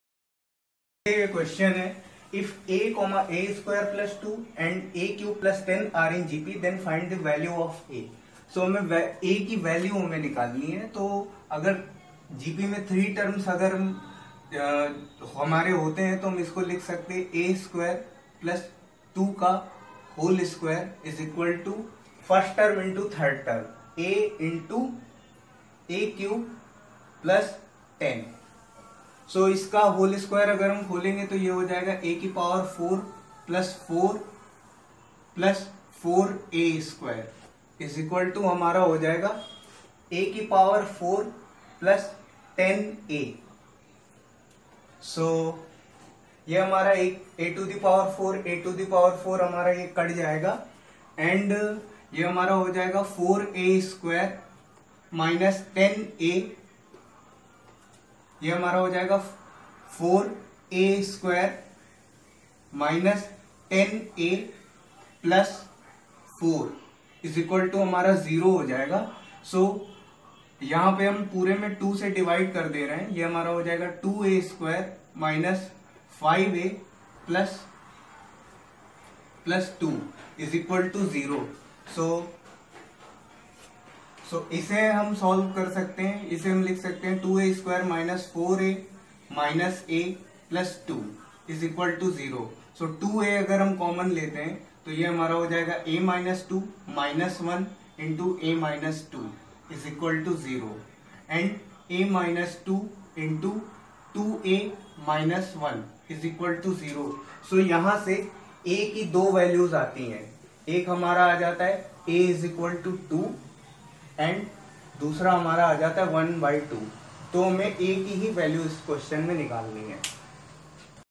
AP & GP Ex9.4 Q9 Class X ICSE | IIT JEE If a, a2+ 2 and a3 + 10 are in G.P., then find the values(s)
If a, a2+ 2 and a3 + 10 are in G.P., then find the values(s) of a.